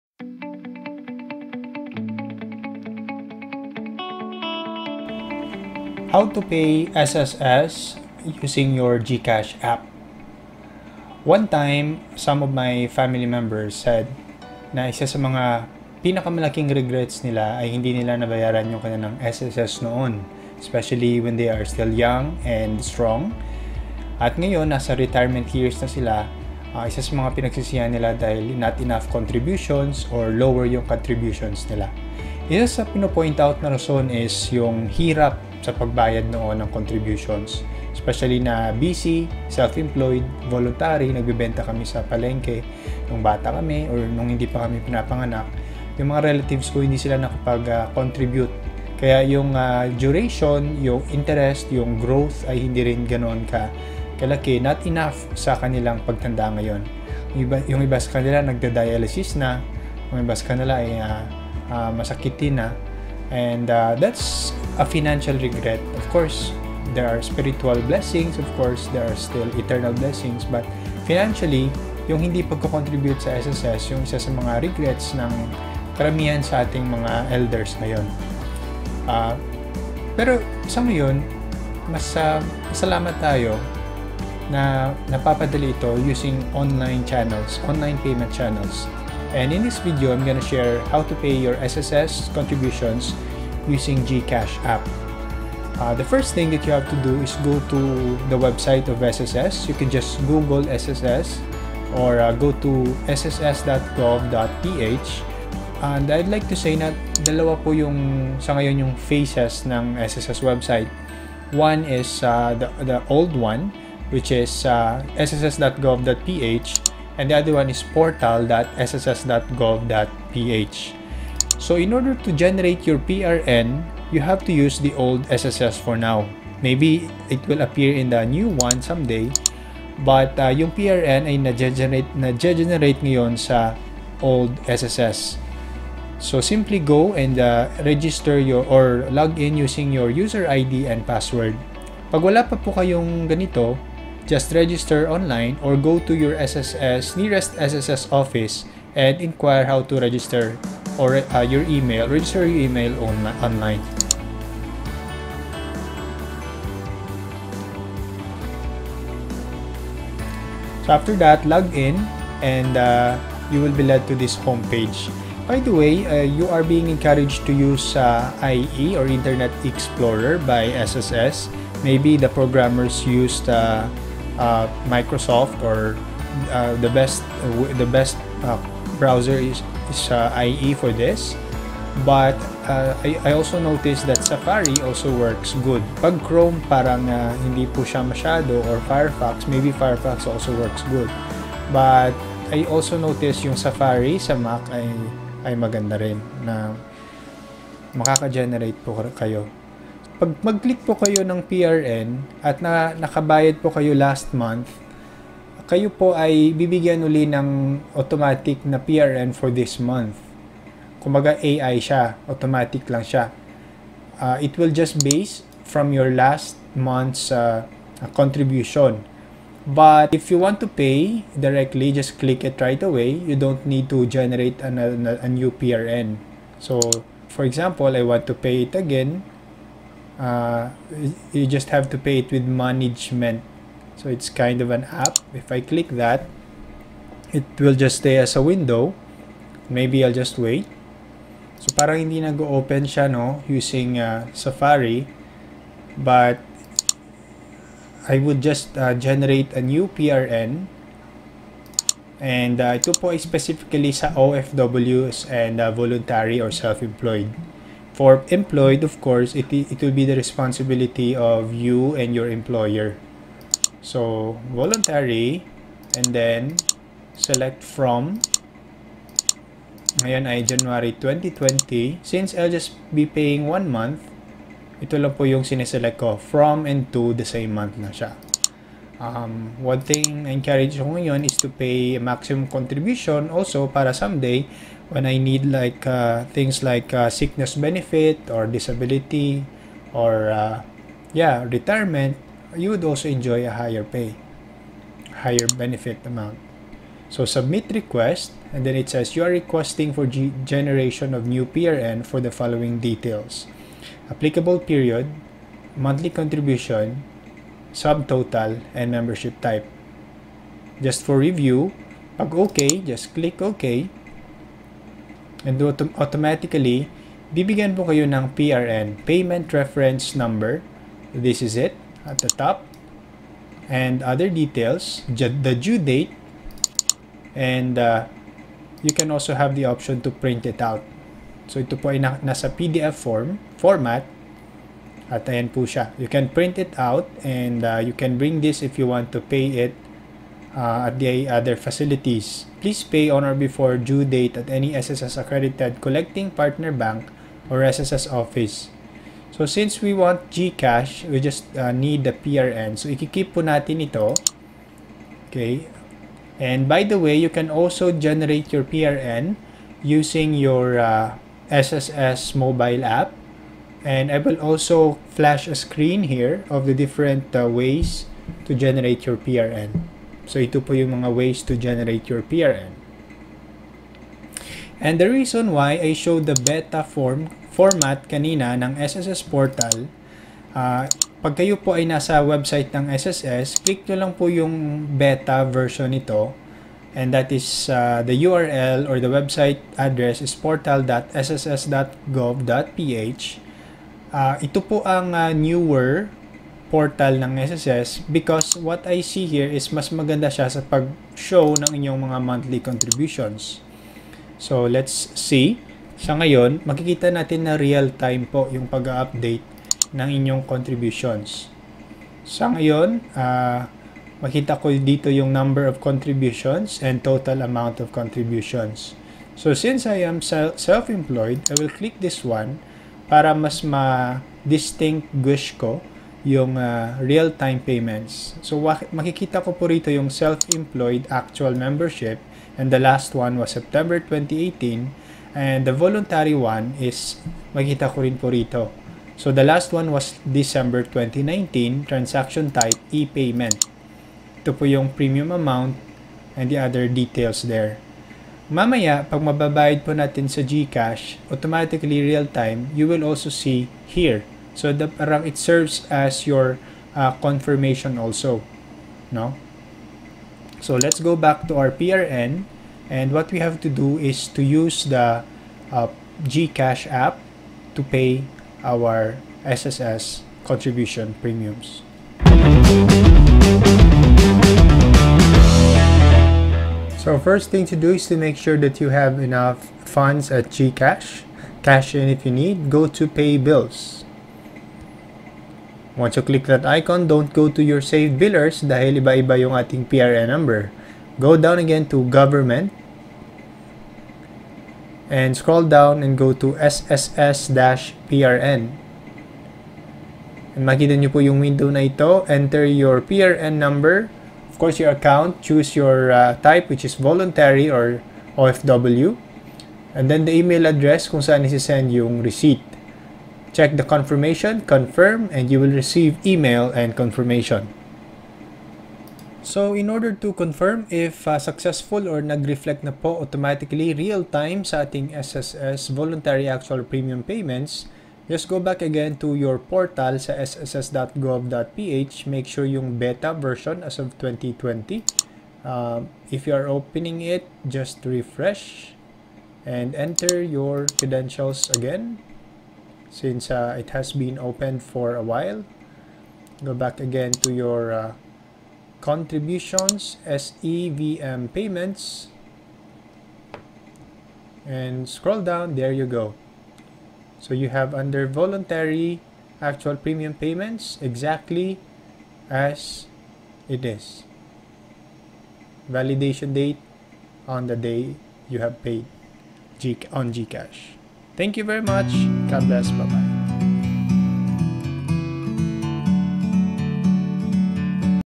How to Pay SSS Contributions via GCash App
Using SSS Website Portal / App, Generate PRN and Pay via GCash App